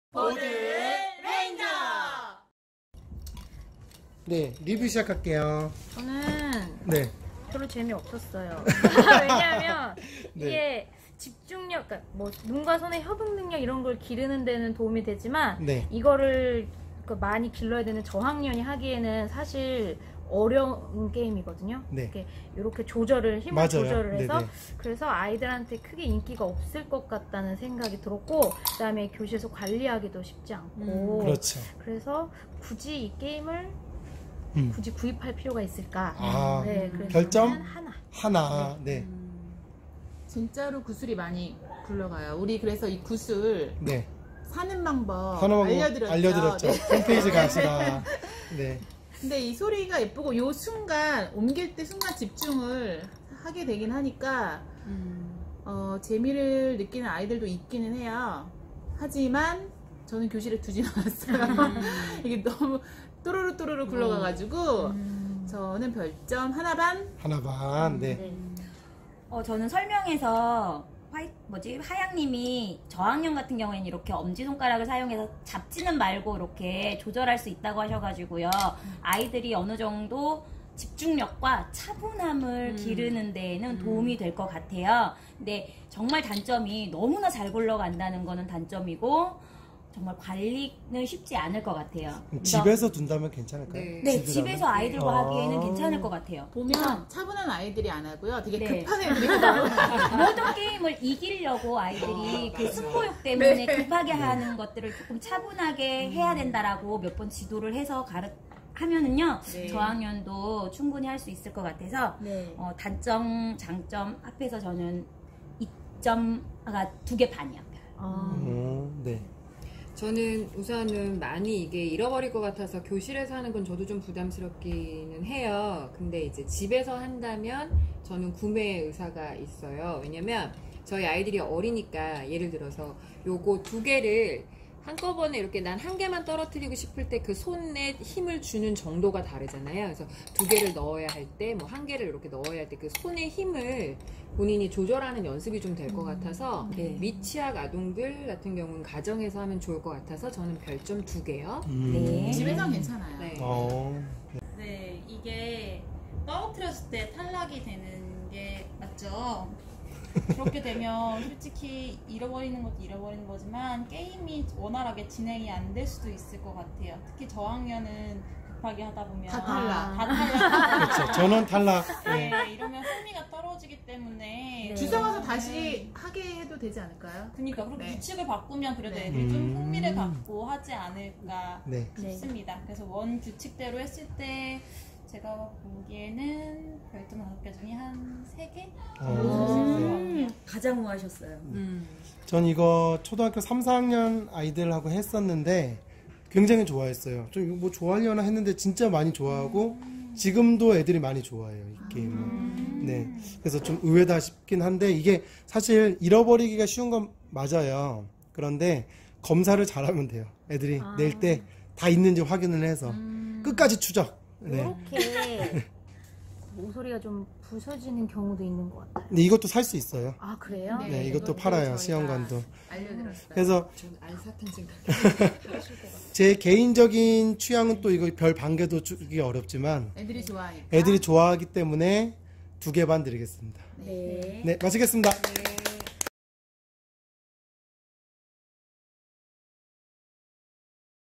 79-2. 닥터 유레카 솔직 리뷰 영상
'구슬이 너무 잘 굴러가서 관리가 어렵다'는 단점이 많이 나왔는데,
게임 상자(아래, 위 2개 있어요)를 아래 깔고 비커를 이리저리 옮기면
구슬이 떨어져도 게임 상자 안에서 굴러다니기 때문에 그나마 괜찮습니다.
심지어 홈페이지에서 '상자'만 팔고 있어요^^

[보드레인저]는 초등학생을 위한 보드게임을 소개하고 있습니다.
+ 게임을 더 재미있게 할 수 있는 팁도 제공하고 있습니다 :-)
틀린 부분이 발견되면 댓글로 꼭~ 알려주세요.